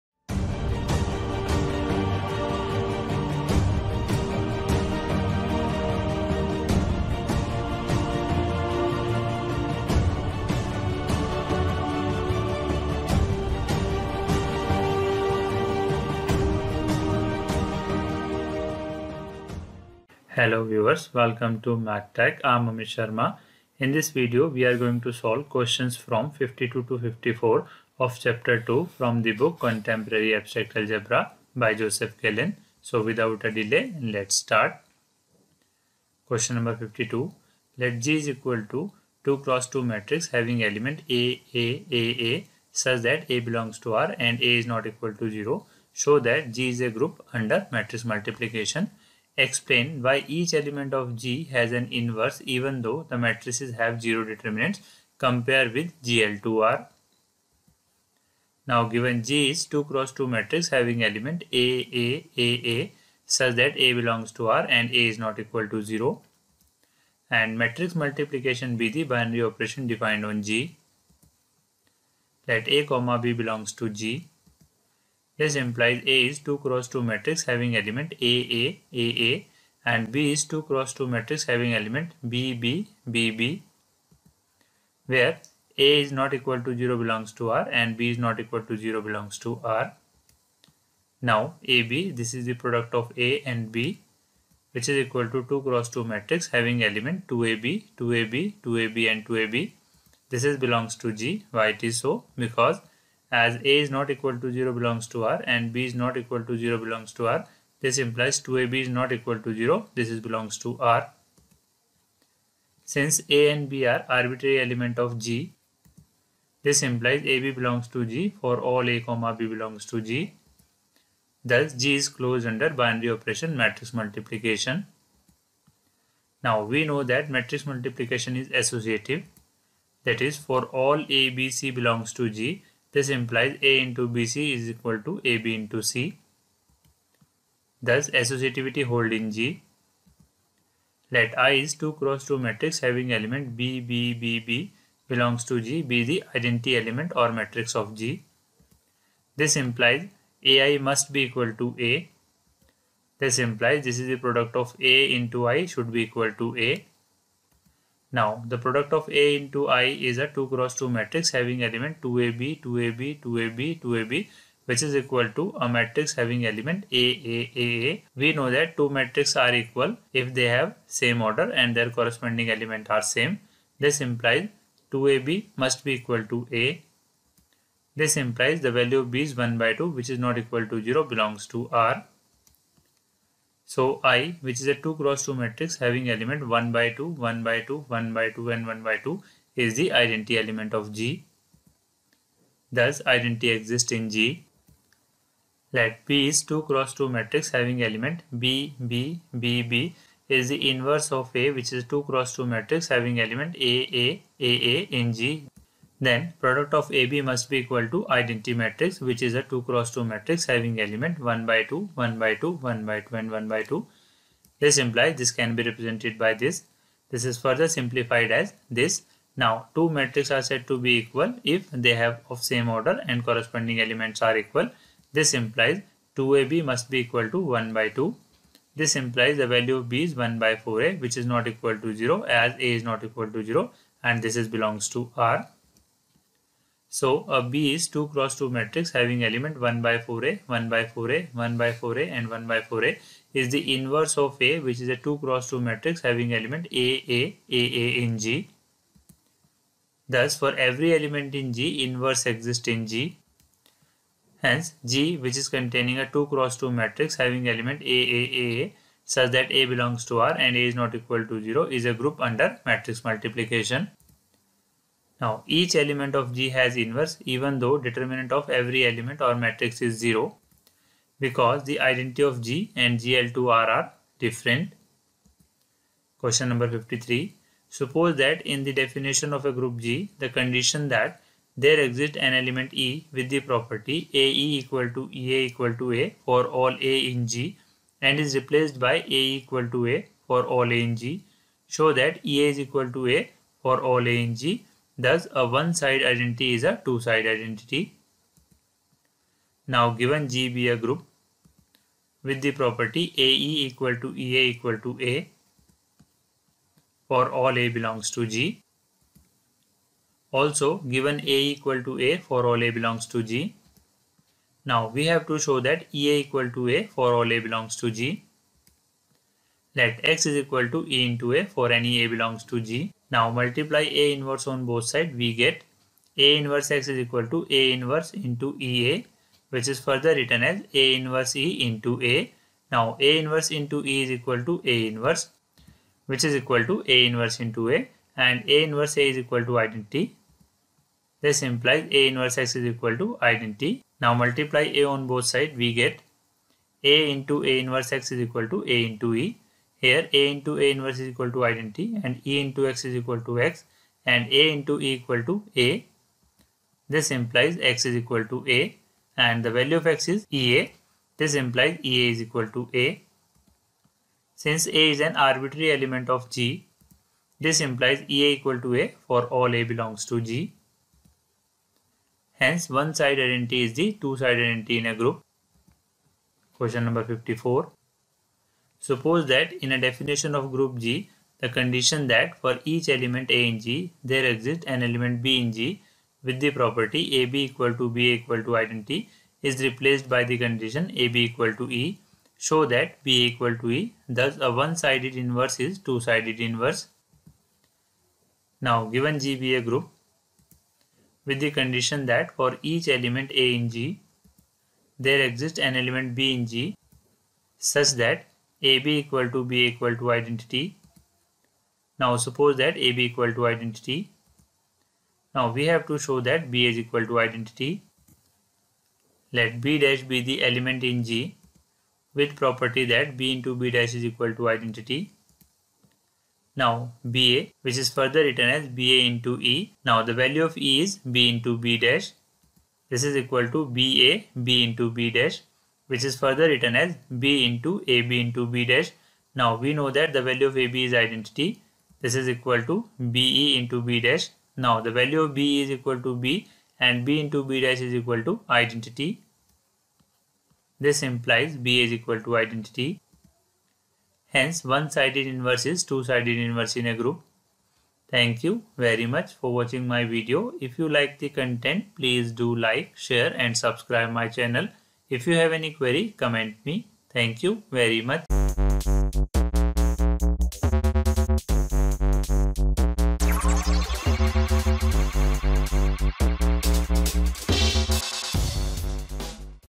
Solution | Que.52-54; Contemporary Abstract Algebra-8th Ed. | Joseph A. Gallian | Chapter-2; Group
In this video we are going to solve Question 52-54 of chapter 2 (Groups) from the book Contemporary Abstract Algebra-8th Ed by Joseph A. Gallian. chapter 2, questions 52-54, Abstract Algebra.